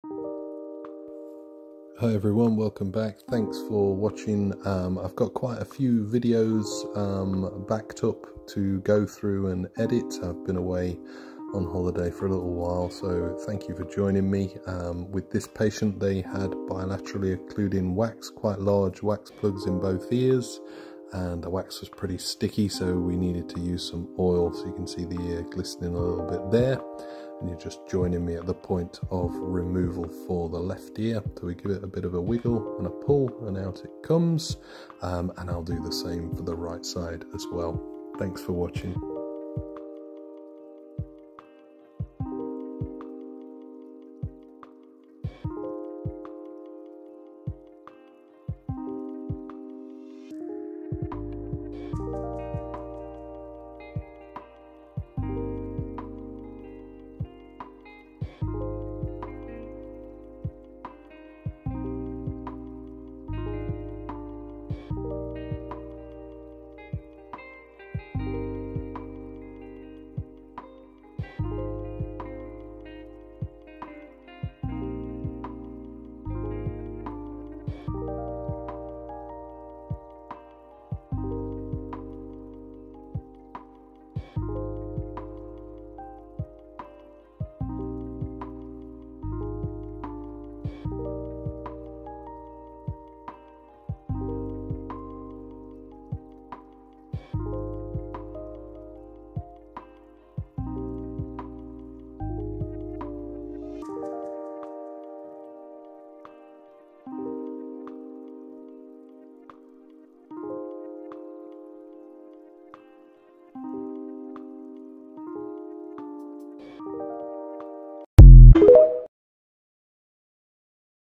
Bilateral wax plugs removed.
Bilateral wax plugs removed using microsuction/otomicroscopy.

Dr. Jeff Davies | Expert Audiologist
Director of Ear Care Specialists, Nottingham